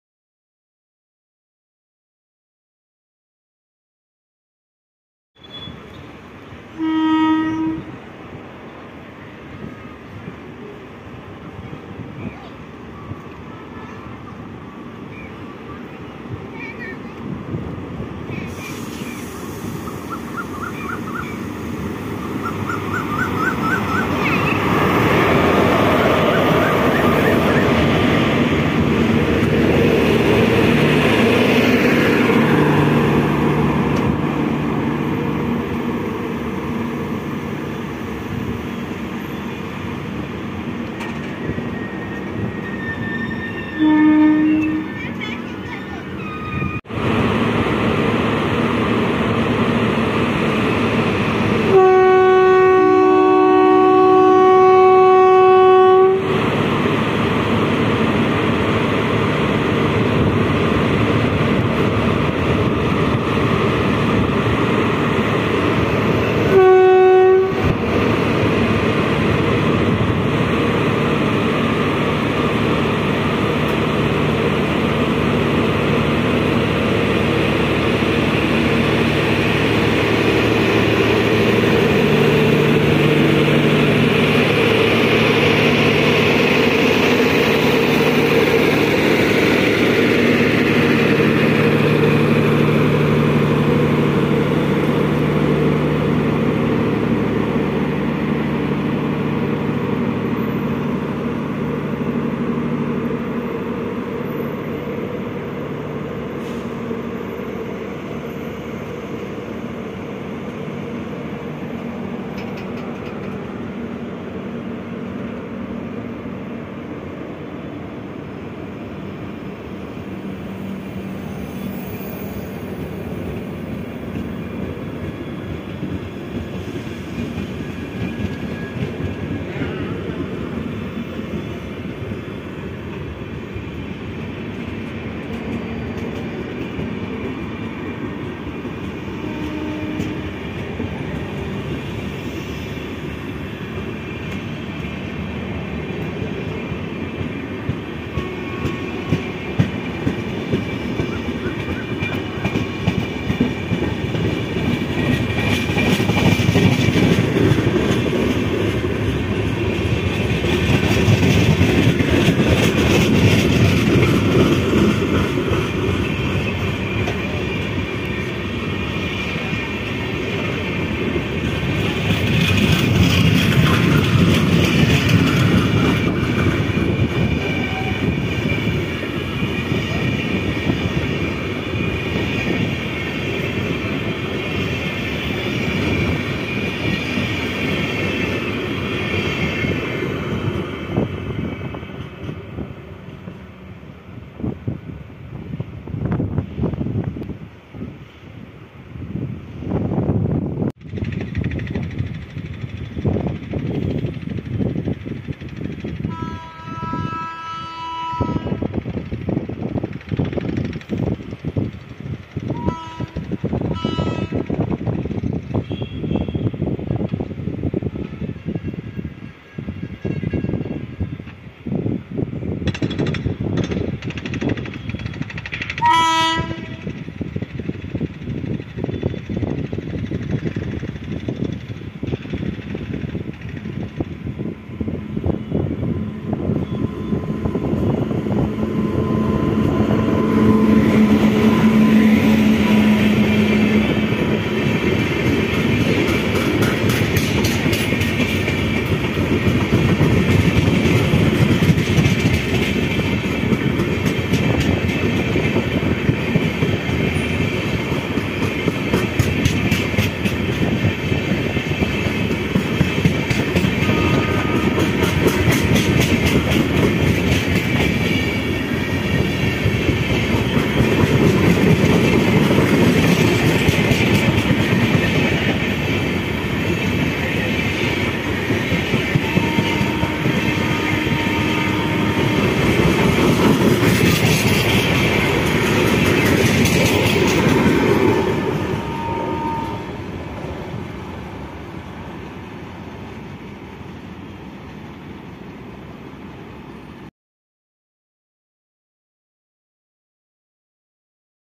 𝐀𝐁𝐑 𝐖𝐀𝐏 4 𝐰𝐢𝐭𝐡 𝐒𝐚𝐮𝐫𝐚𝐬𝐡𝐭𝐫𝐚 𝐦𝐚𝐢𝐥 || 𝐁𝐑𝐂 𝐖𝐀𝐏 7 𝐰𝐢𝐭𝐡 𝐉𝐚𝐢𝐩𝐮𝐫 𝐎𝐤𝐡𝐚 𝐰𝐞𝐞𝐤𝐥𝐲 𝐞𝐱𝐩𝐫𝐞𝐬𝐬
𝐓𝐇𝐄 𝐎𝐅𝐅𝐋𝐈𝐍𝐊 ❗

1) 22946 𝗢𝗸𝗵𝗮 𝗠𝘂𝗺𝗯𝗮𝗶 𝗰𝗲𝗻𝘁𝗿𝗮𝗹 𝗦𝗮𝘂𝗿𝗮𝘀𝗵𝘁𝗿𝗮 𝗺𝗮𝗶𝗹 𝘄𝗶𝘁𝗵 22612 𝗔𝗕𝗥 𝗪𝗔𝗣 𝟰 ❤

2) 20952 𝗝𝗮𝗶𝗽𝘂𝗿 𝗢𝗸𝗵𝗮 𝘄𝗲𝗲𝗸𝗹𝘆 𝗲𝘅𝗽𝗿𝗲𝘀𝘀 𝘄𝗶𝘁𝗵 39080 𝗕𝗥𝗖 𝗪𝗔𝗣 𝟳 𝘀𝗸𝗶𝗽𝗽𝗶𝗻𝗴 𝗠𝗶𝘁𝗵𝗮𝗽𝘂𝗿 ⚡

📍𝙇𝙤𝙘𝙖𝙩𝙞𝙤𝙣 - 𝙈𝙞𝙩𝙝𝙖𝙥𝙪𝙧 𝙧𝙖𝙞𝙡𝙬𝙖𝙮 𝙨𝙩𝙖𝙩𝙞𝙤𝙣, 𝙂𝙪𝙟𝙖𝙧𝙖𝙩

𝘿𝙖𝙮 - 𝙒𝙚𝙙𝙣𝙚𝙨𝙙𝙖𝙮